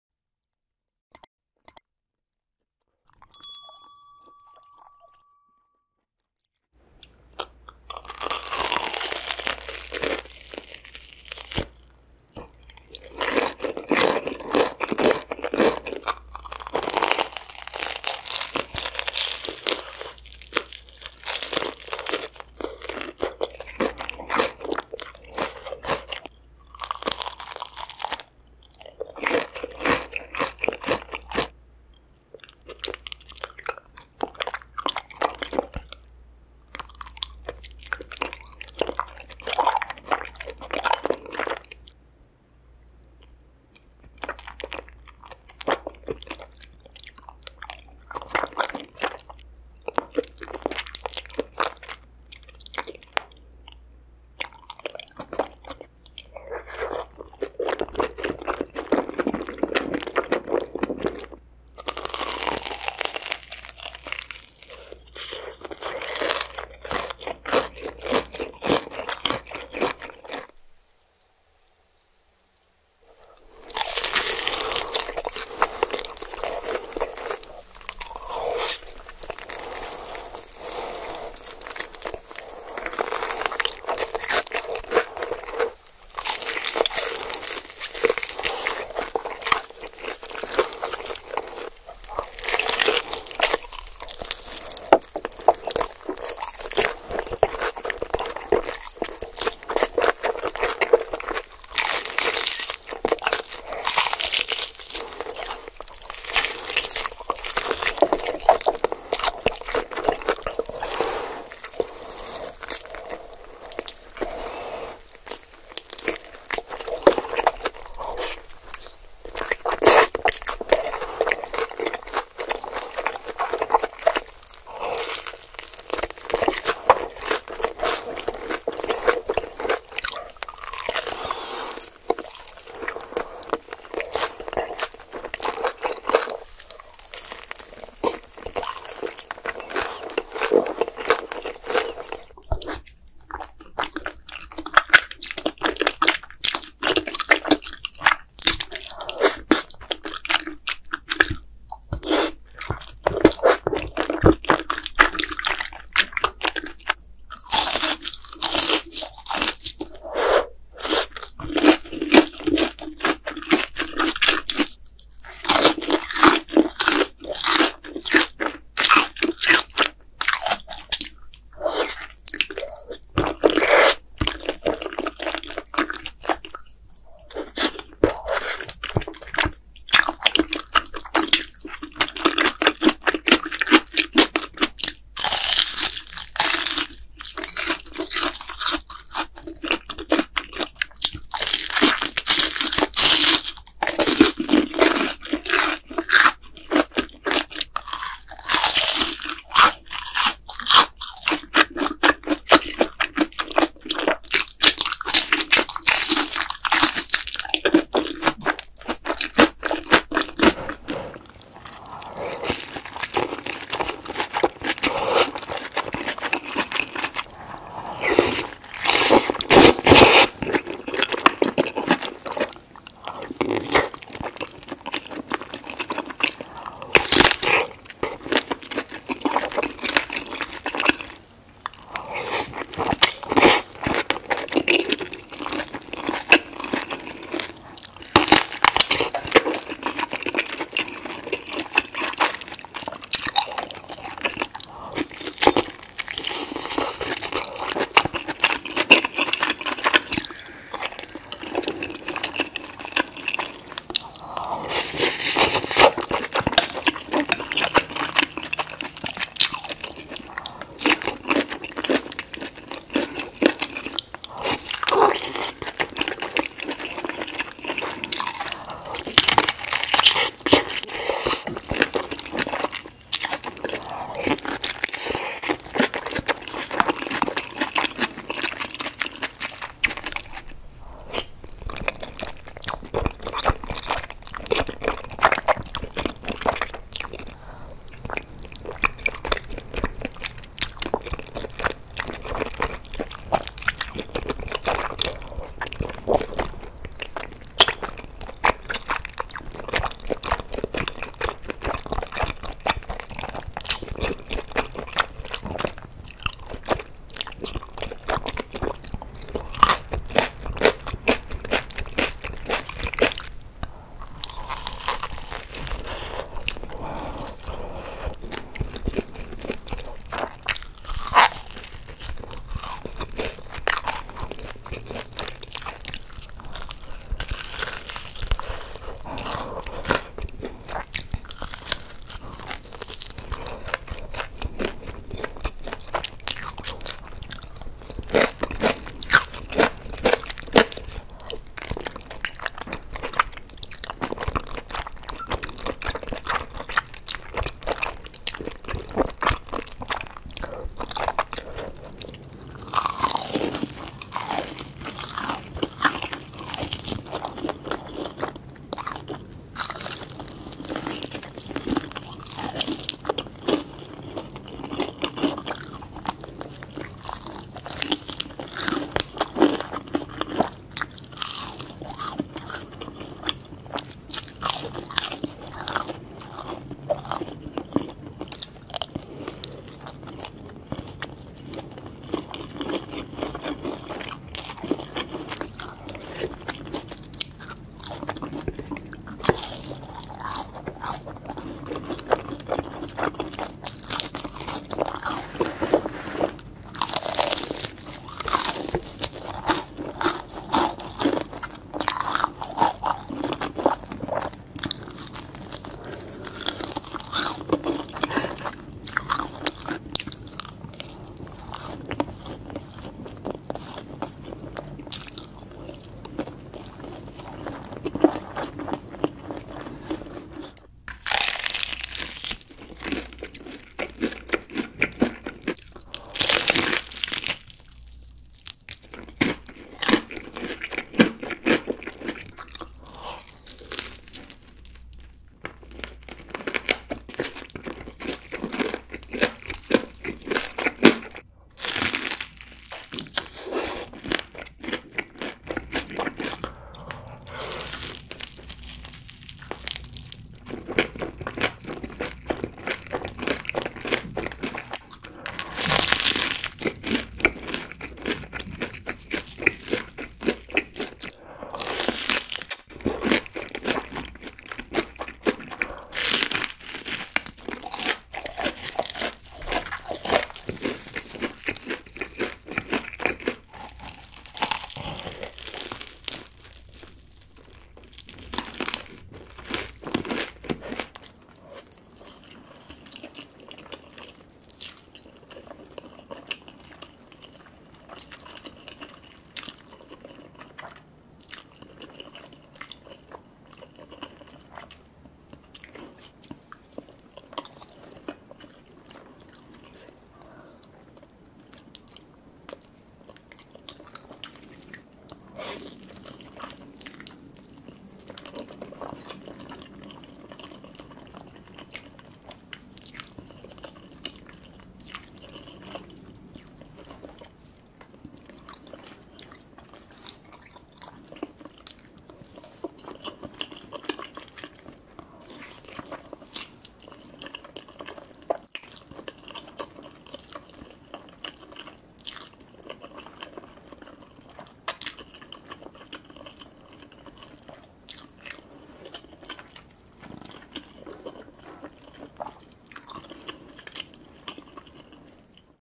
Compilation Asmr Eating - Mukbang, Phan, Zach Choi, Jane, Sas Asmr, ASMR Phan, Hongyu | Part 604
Today in the compilation: CARBO FIRE NOODLES, HONEY GARLIC WINGS, CORN CHEESE, KING CRAB, CORN DOG, TTEOKBOKKI, FRIED CHICKEN, FIRE NOODLES, SEAFOOD
까르보불닭볶음면, 꿀 마늘 날개, 옥수수 치즈, 킹크랩 먹방, 치즈 핫도그, 떡볶이, 프라이드 치킨, 불닭볶음면, 해산물